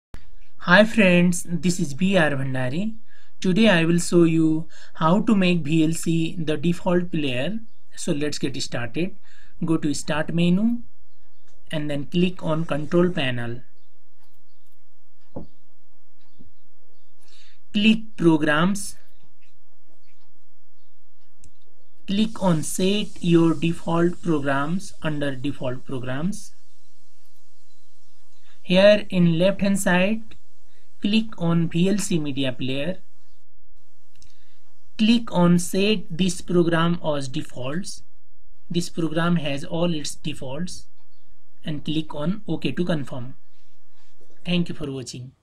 How to Make VLC The Default player In Windows 7
This short tutorial will show you how to make vlc the default player